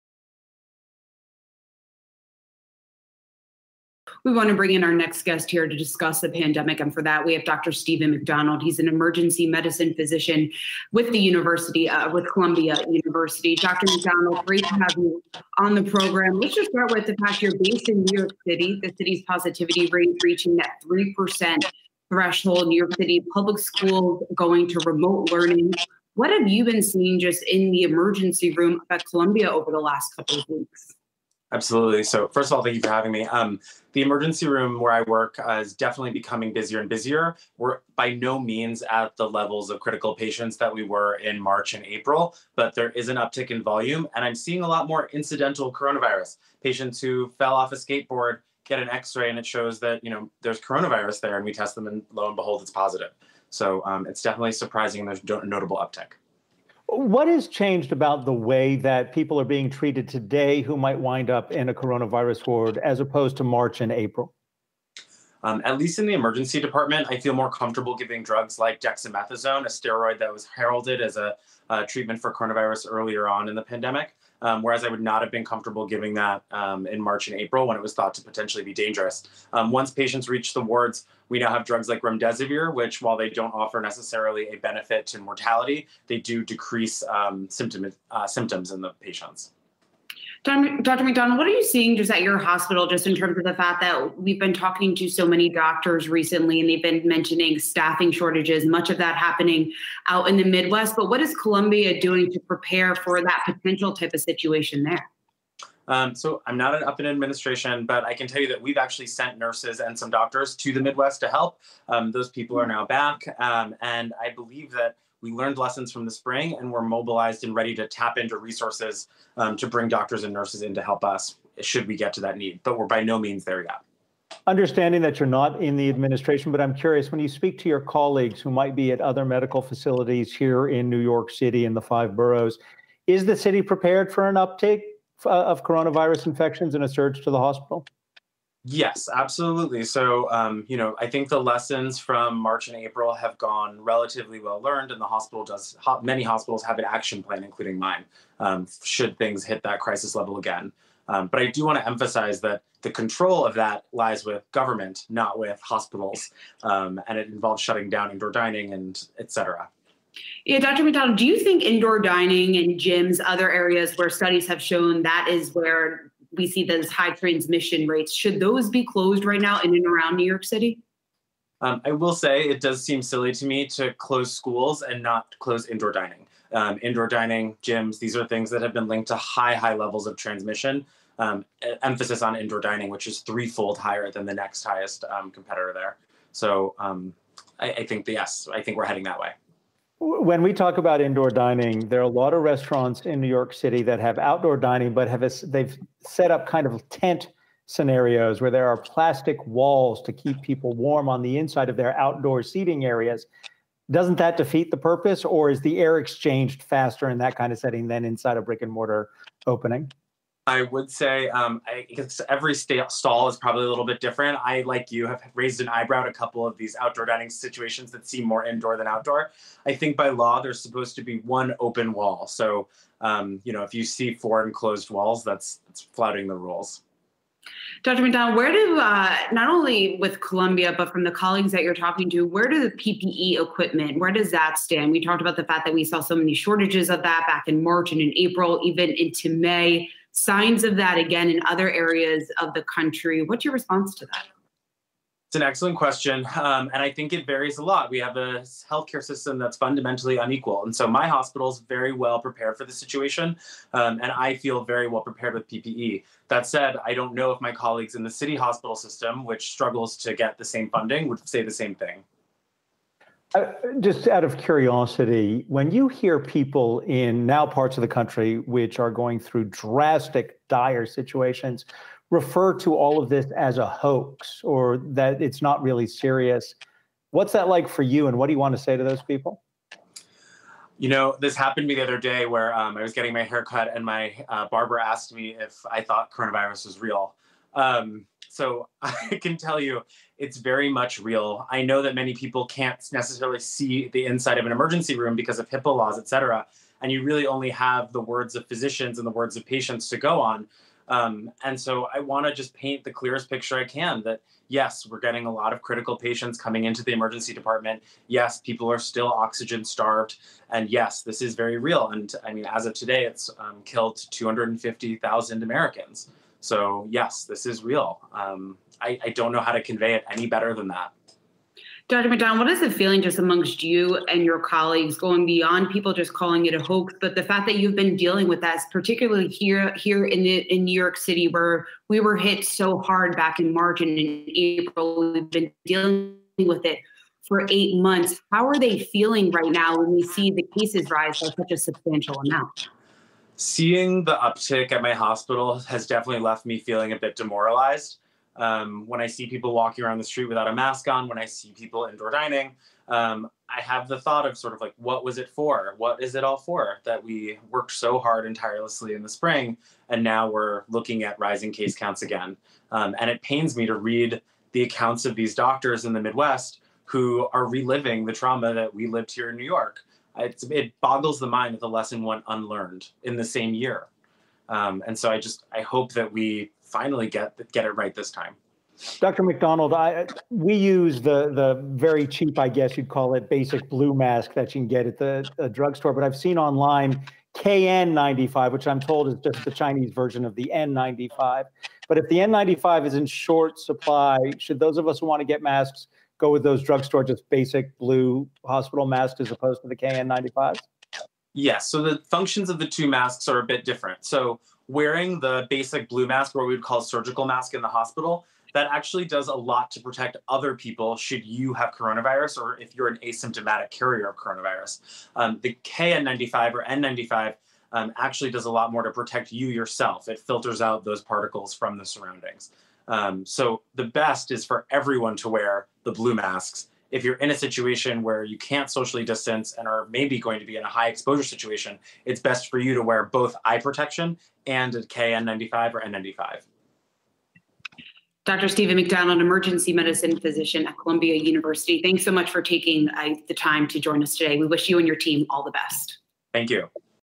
Coronavirus deaths in the United States top 250,000: 'This is very real,' says NYC ER physician
Emergency Medicine Physician Dr. Steven McDonald weighs in on the uptick in coronavirus cases across the country, in New York City, and the impact on medical staff and hospitals.